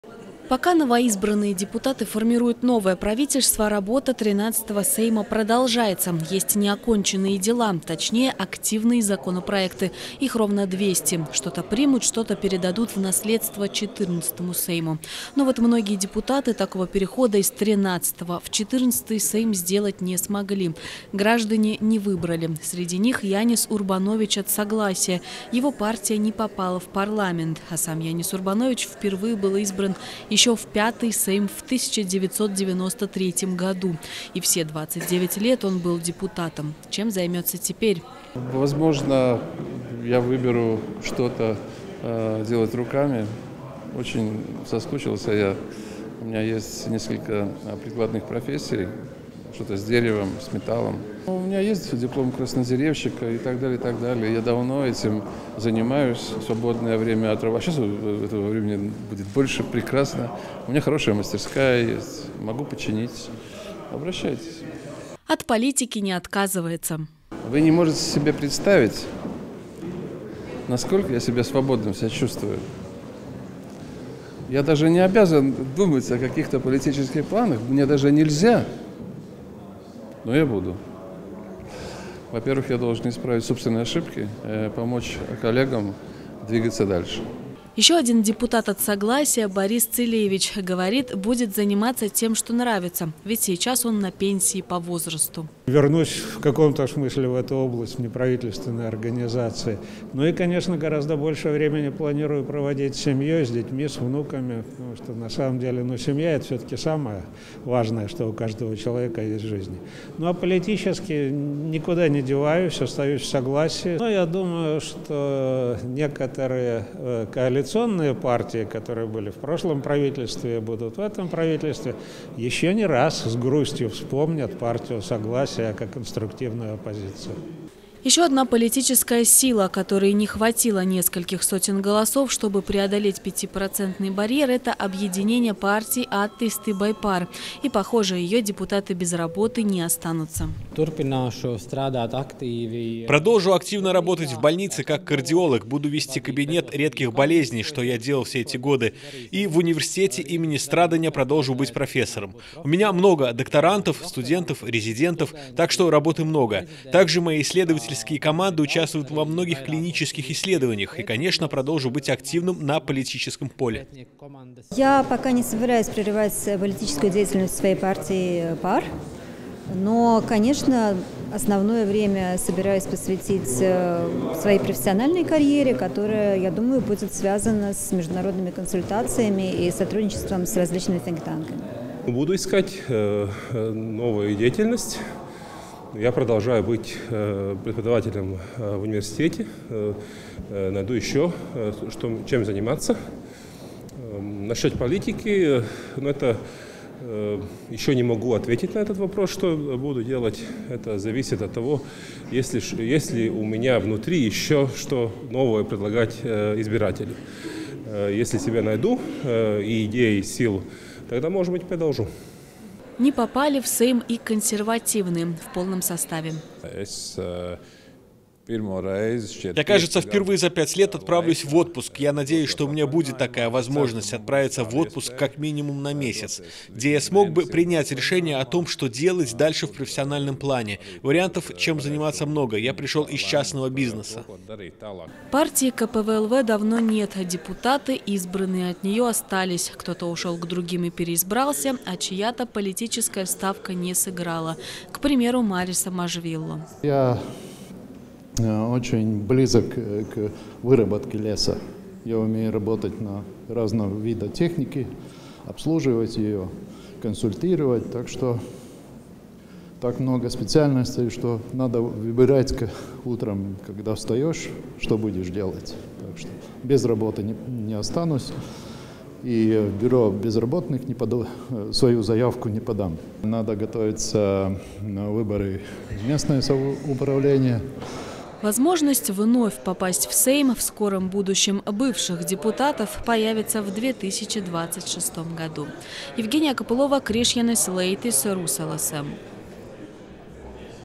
Есть ли жизнь после Сейма?
Есть ли жизнь после Сейма? Многие депутаты скоро закончат свою работу в парламенте. Некоторые из них работали депутатами без малого три десятилетия. Чем будут заниматься теперь? Евгения Копылова отправилась к законодателям, чтобы спросить об их творческих и не только планах.